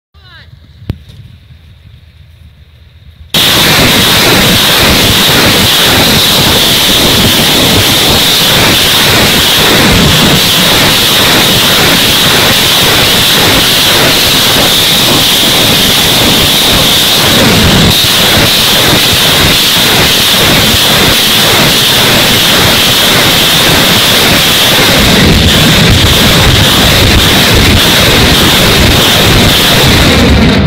БМ-21, «Град», залп ночью 06.02.2015.
Залп из "Градов" (БМ-21) шестого февраля 2015 года. Луганская область.